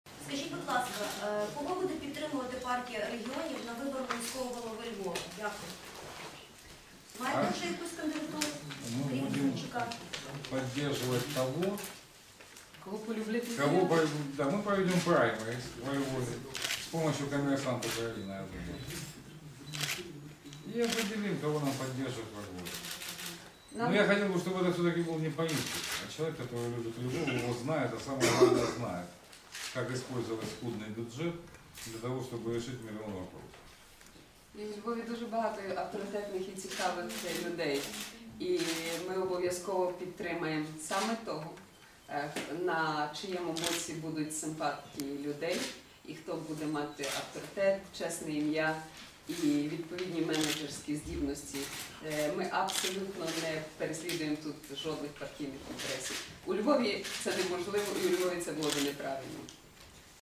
Колесников про підтримку львівського мера
Прес-конференція народних депутатів від Партії регіонів Бориса Колесникова та Ганни Герман у Львові.